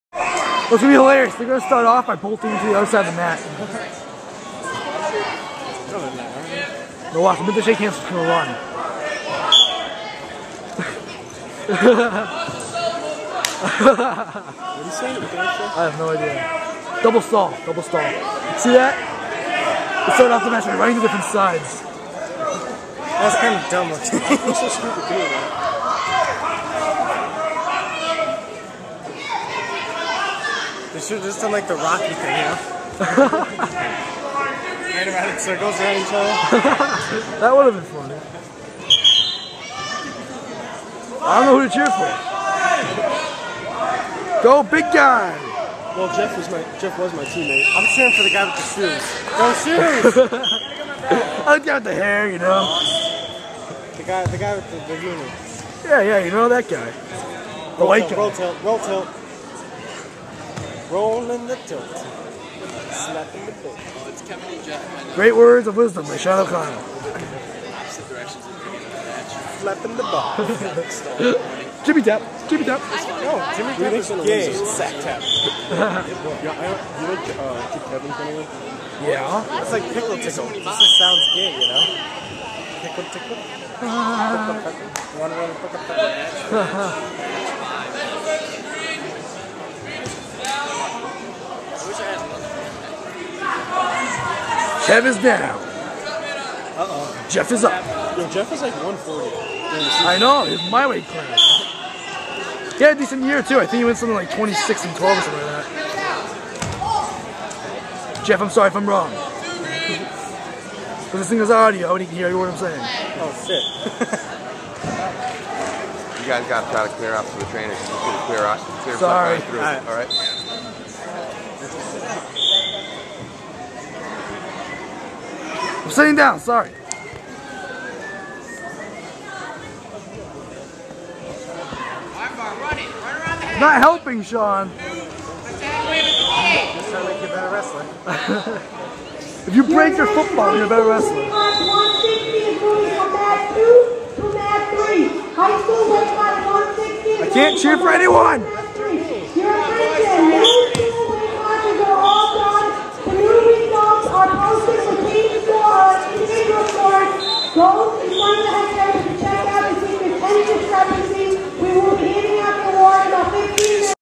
Kevin vs Jeff
Death Knights have to wrestle...boo. Watch what we do in the beginning. The other refs were fun :/. I hate to wrestle a team mate but, meh